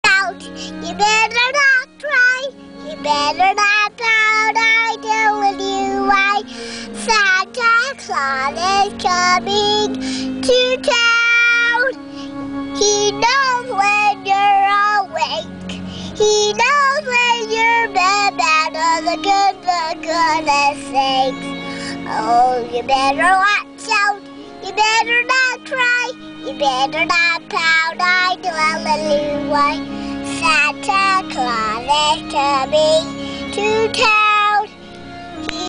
Calla Lily Sings !
34 month old Calla Lily sings a christmas song...